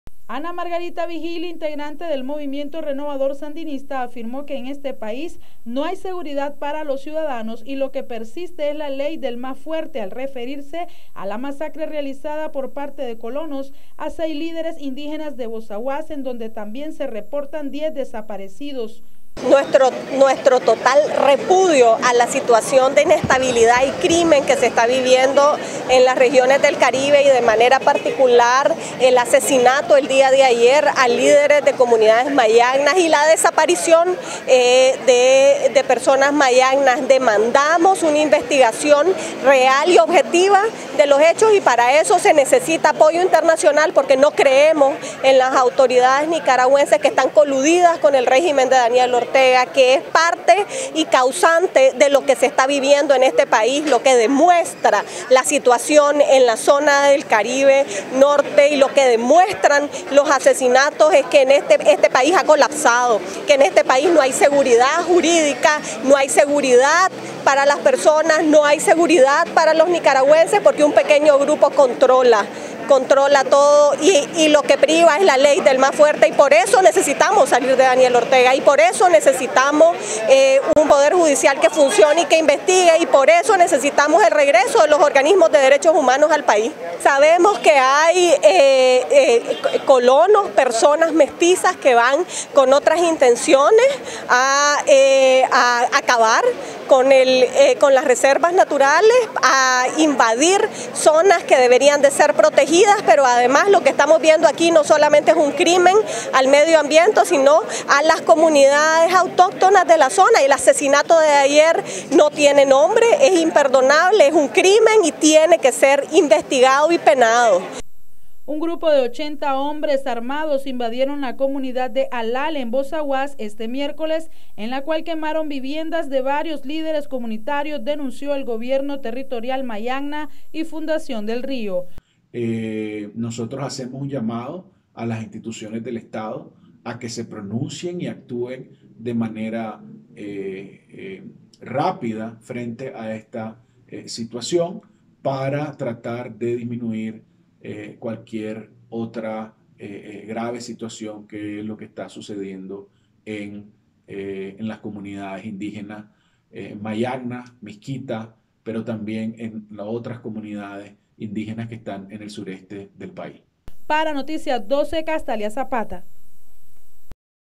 ¨EN ESTE PAÍS PREVALECE LA LEY DEL MÁS FUERTE¨
En este país prevalece la ley del más fuerte, dijo Ana Margarita Vijil.